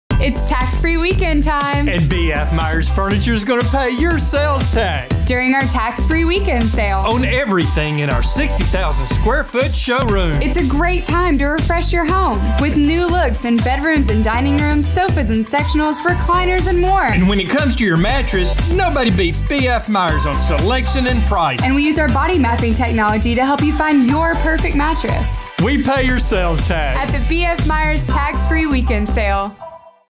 BF Myers Tax Free Sale 2019 30
We'll pay your sales tax on everything in the store this weekend!